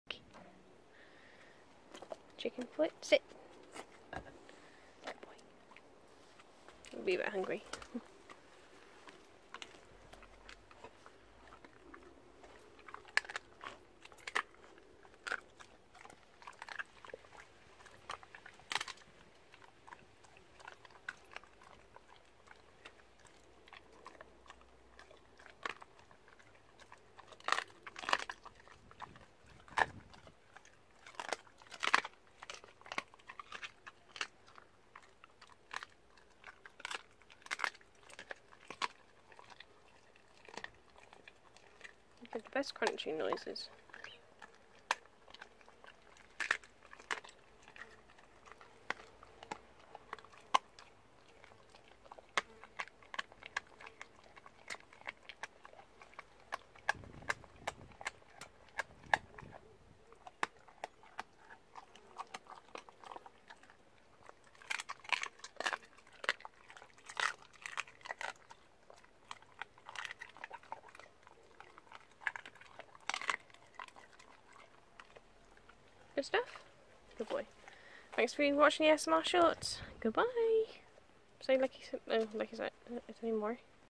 ASMA SHORTS: Spaniel crunching on chicken foot.
Hey guys! I really hope your enjoying theses ASMA Shorts as I am, I love watching the dogs eating there raw food. X